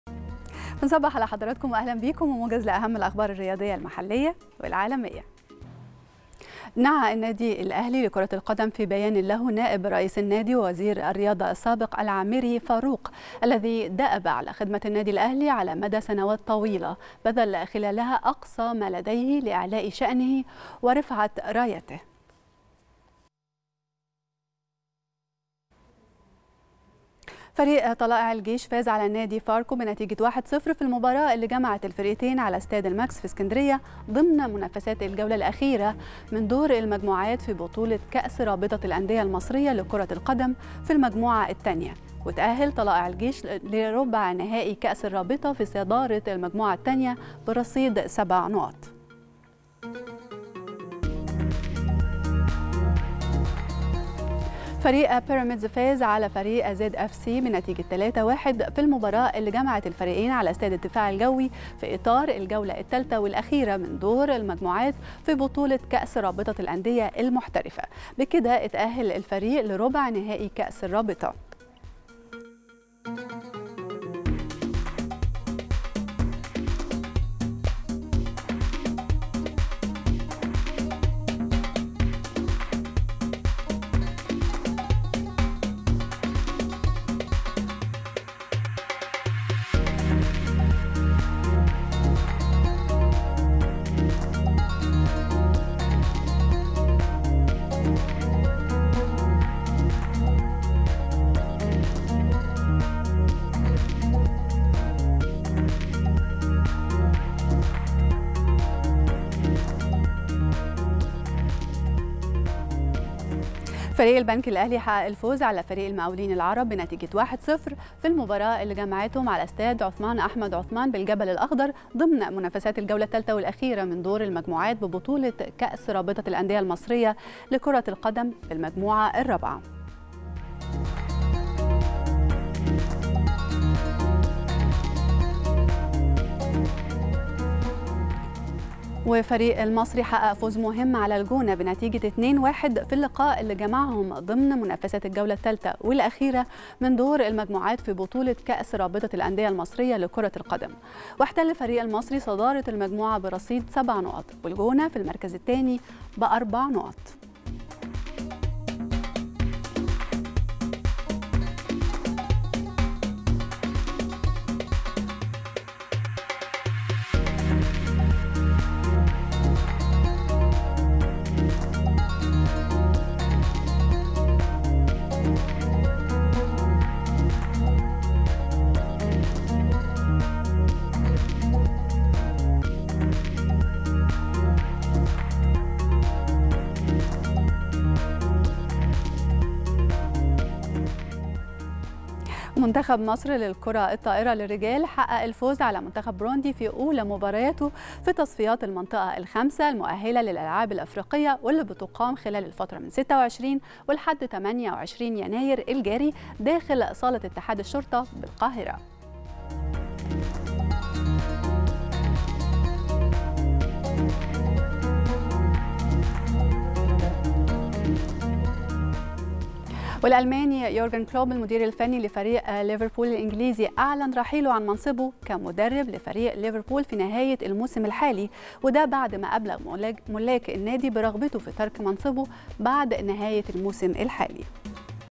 كلوب أعلن رحيله عن تدريب ليفربول نهاية الموسم
صباح الخير يا مصر | كلوب أعلن رحيله عن تدريب ليفربول نهاية الموسم